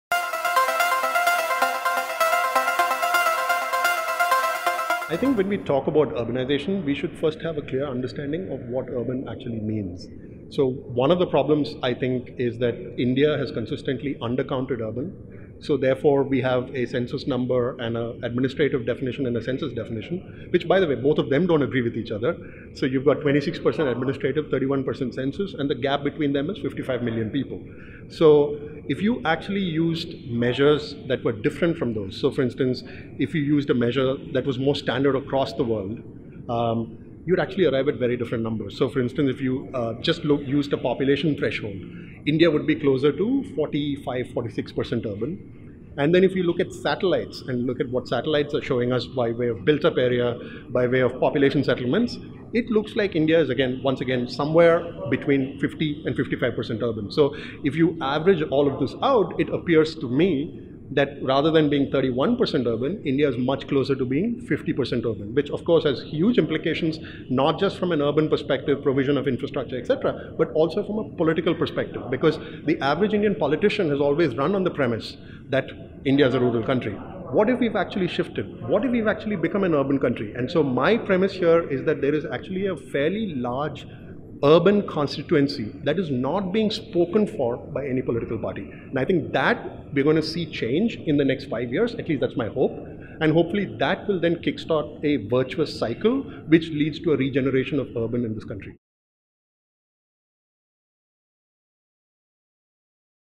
Transformation Agenda 2020: In conversation with Reuben Abraham
‘In conversation with Reuben Abraham, CEO, IDFC Institute at the ‘Transformation Agenda 2020’ conference on India’s changing urban landscape held in Mumbai on 8th June, organised jointly by Mint and Danfoss. Key speaker, Nitin Gadkari, Minister of Road Transport and Highways spoke at the event. The theme 'India's changing urban landscape' will deep dive into the opportunities and challenges faced